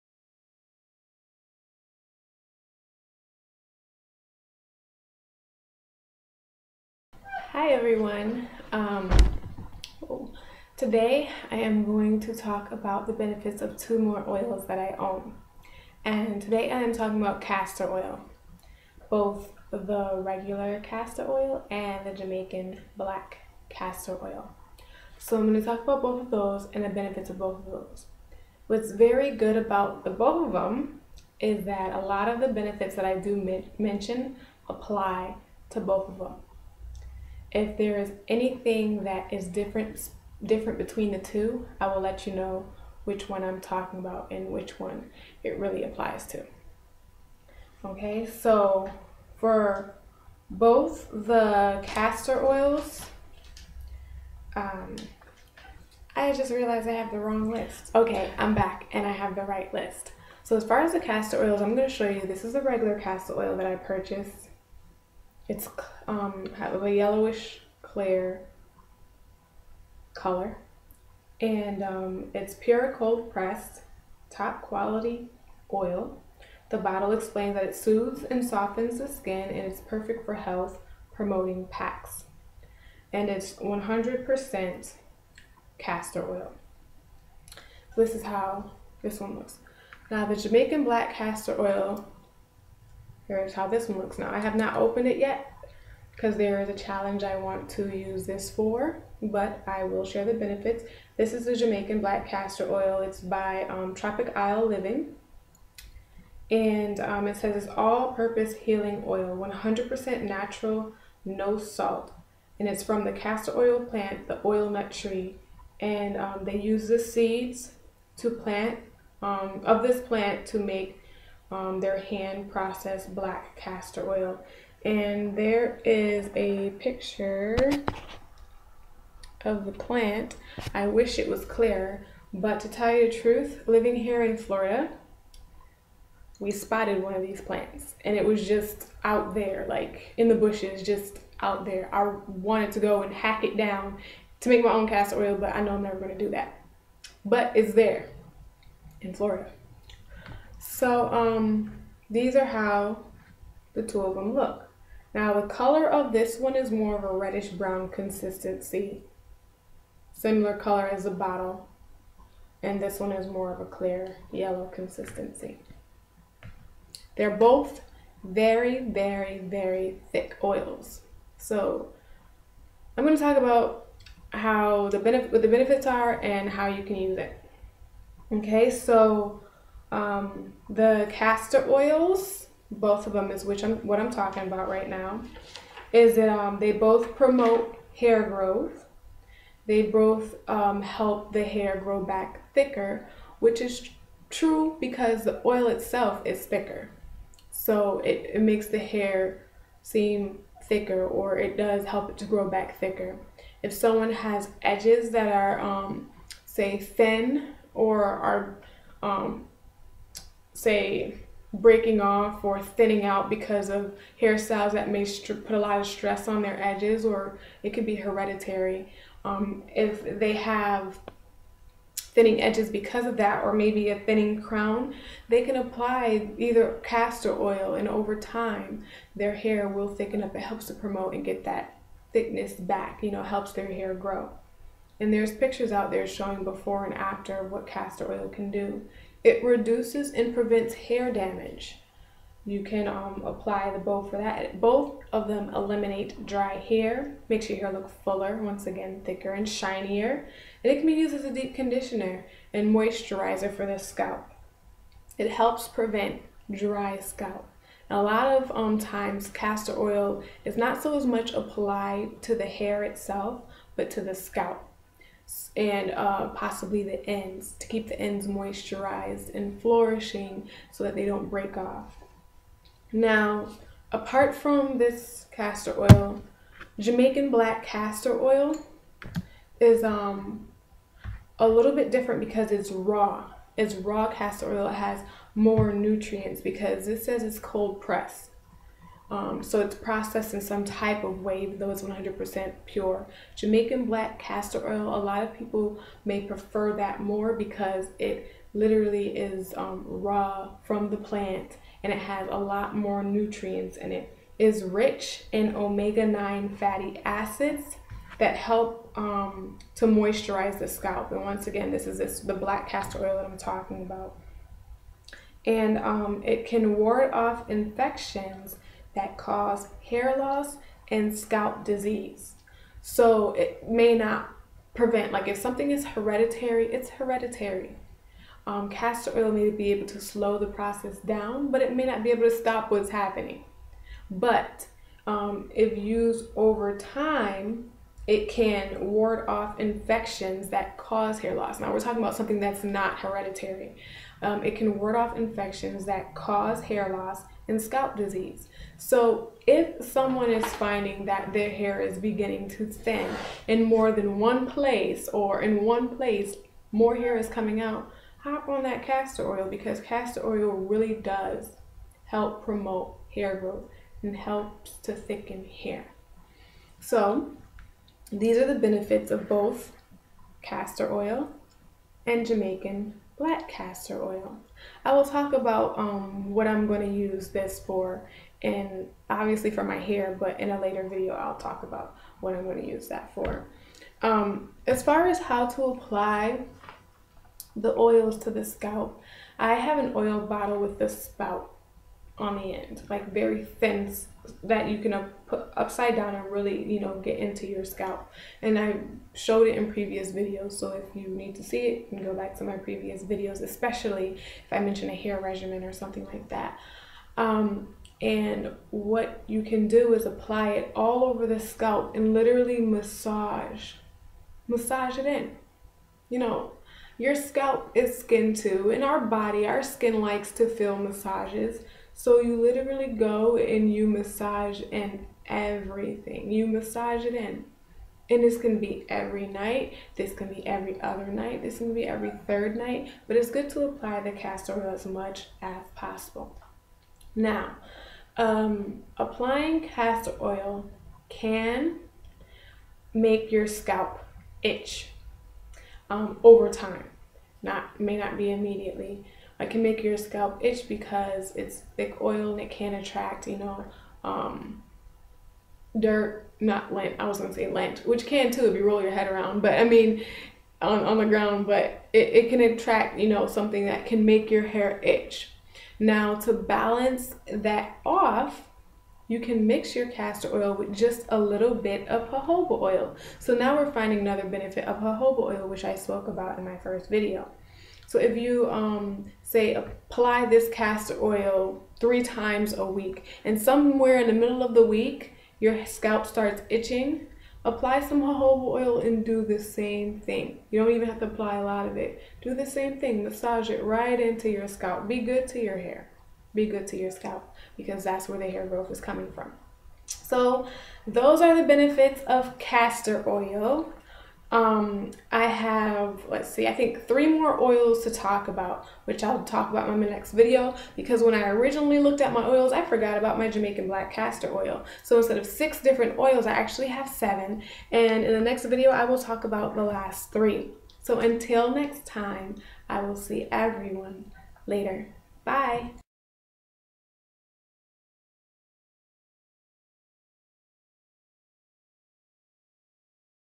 Benefits of Castor Oil & Jamaican Black Castor Oil
In this vid I explain the benefits of castor oil & jamaican black castor oil. Enjoy!

Here's my last vid on the benefits of olive oil and jojoba oil if you haven't seen it yet: